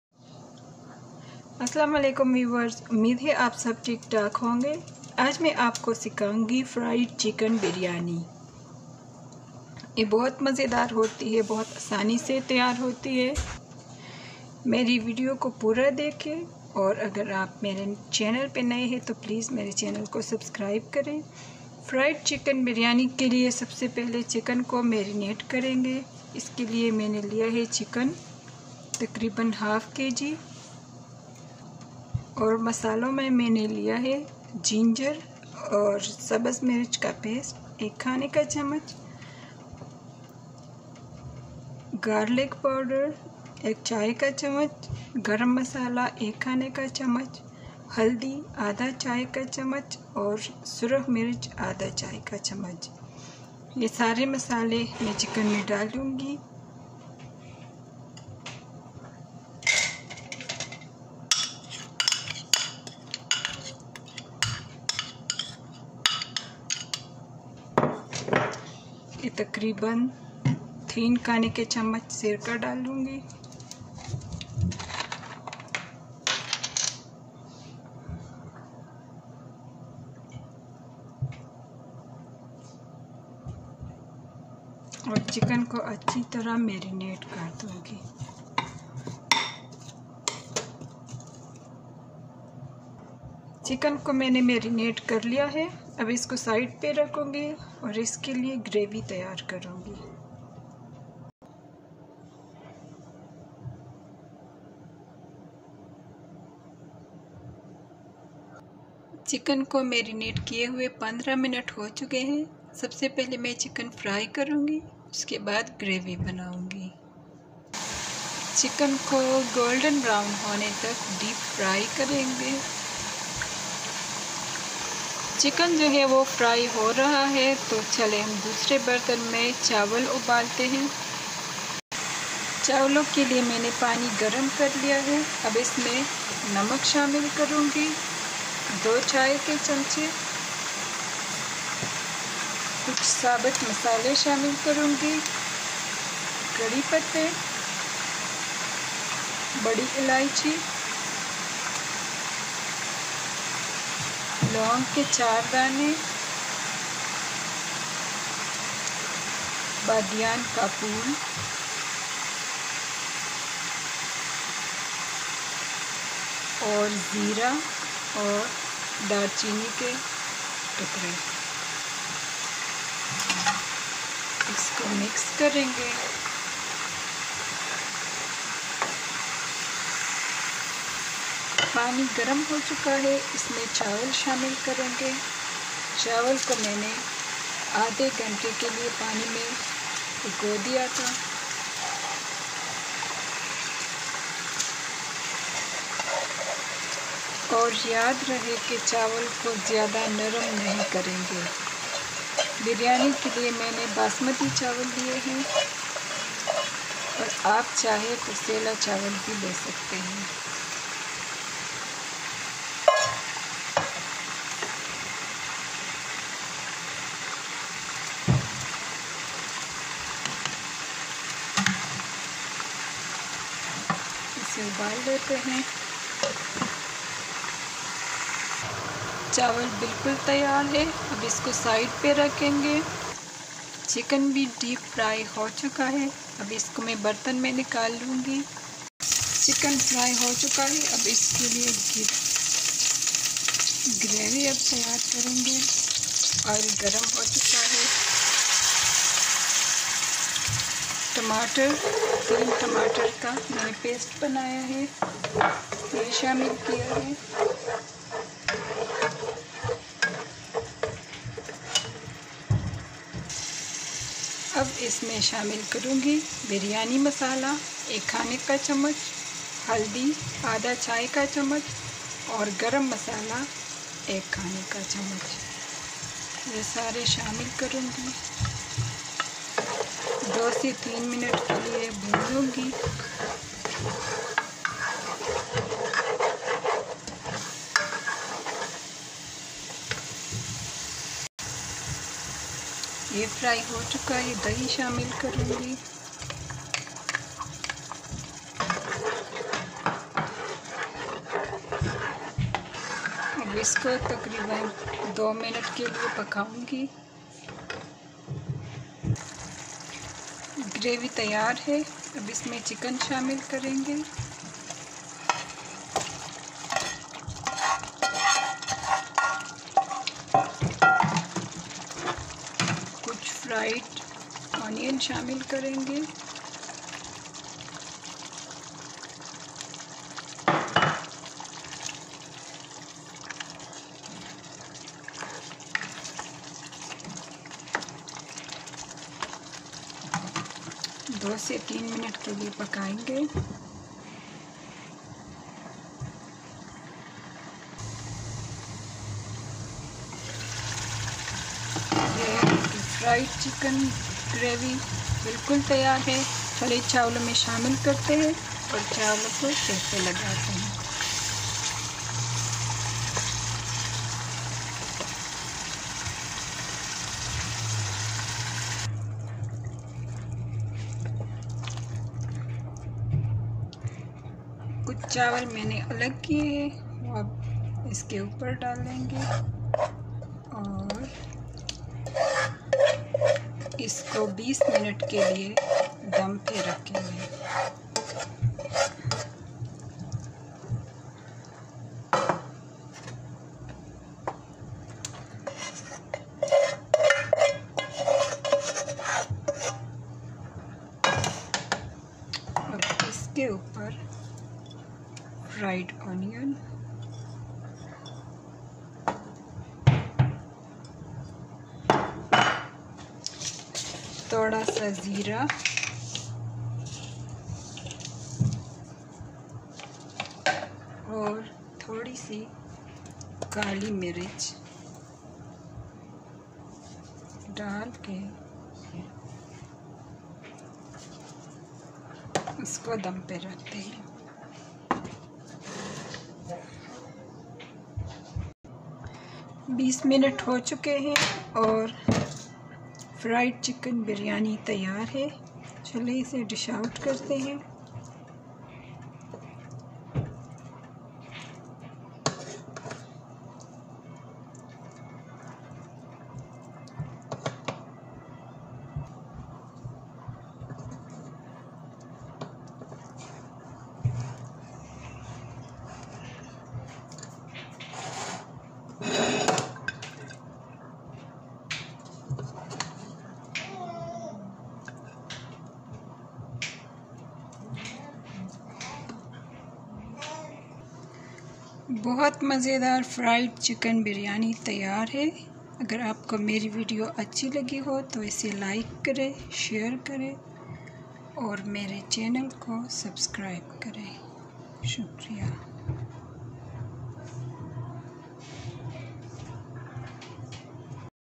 Fried Chicken Biryani Recipe | Tasty Chicken Biryani
This is a very easy and yummy process of cooking Fried Chicken Biryani Recipe.
Ingredients :
Chicken 1/2 kg (1lb).
Rice 2 cups (soaked for 30 minutes).
Chicken marination :
Ginger and green chili paste 1tbsp.
Garlic powder 1tsp.
Salt 1tsp.
Turmaric 1/2 tsp.
Vinegar 3tbsp.
Oil for chicken deep fry.
For chicken gravey:
Oil 1/2 cup.
Fried onions 1 cup or 1 large onion.
3 Tomatoes paste.
Biryani masala 1tbsp.
Turmaric 1/2 tsp.
Sabar garam masala for rice :
Cloves 4 count.
Badyan 1 flower.
Black cardamom 1count.
Cumin 1tsp.
Tez pat 1.
After you have followed all the process in the video, a tasty and easy Fried Chicken Biryani Recipe will be ready to serve.